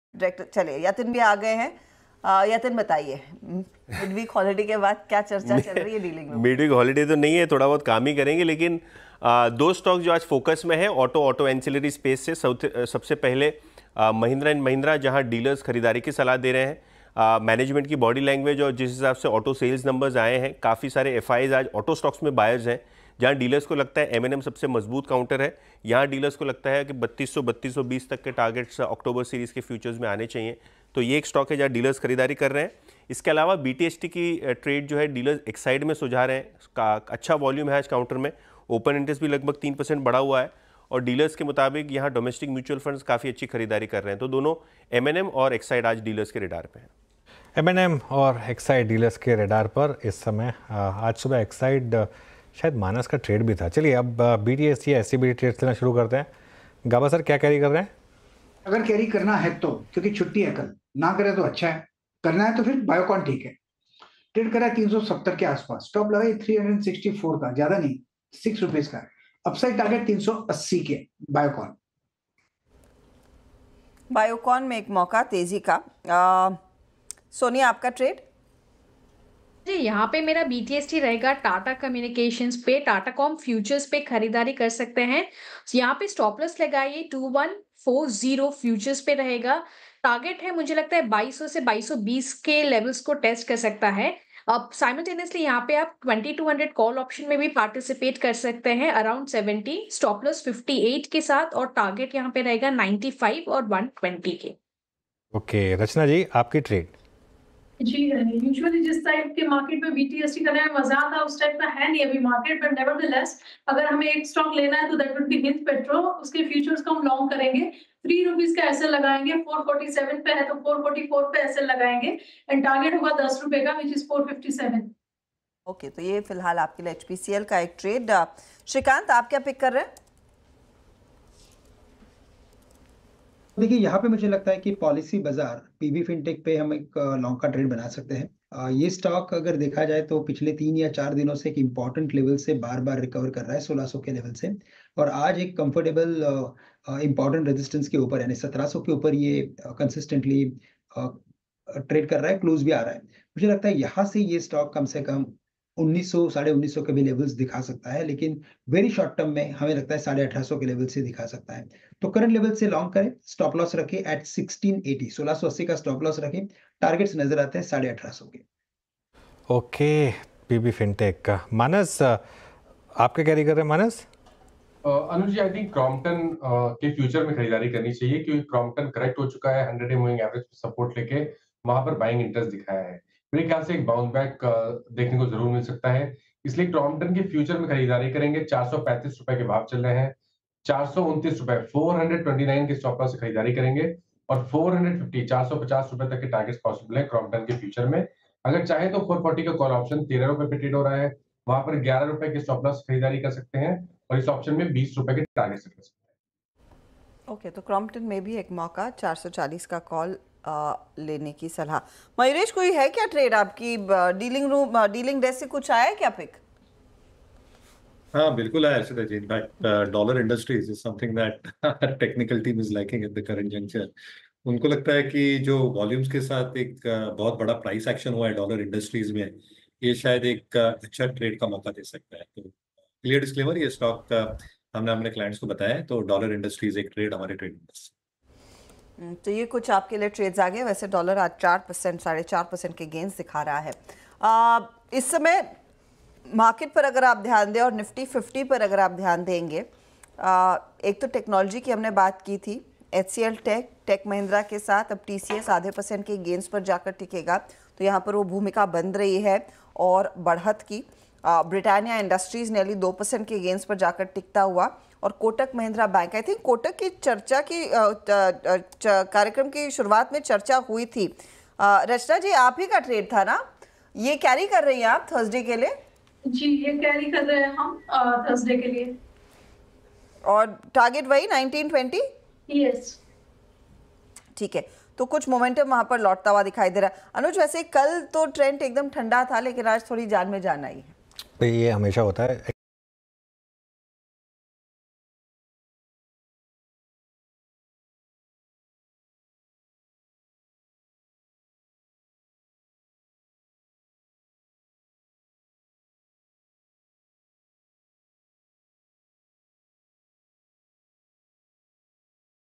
Dealing Room Check: डीलर्स ने इन दो स्टॉक्स में कराई बंपर बाईंग, शॉर्ट टर्म में मिलेगा जोरदार मुनाफा
n18oc_business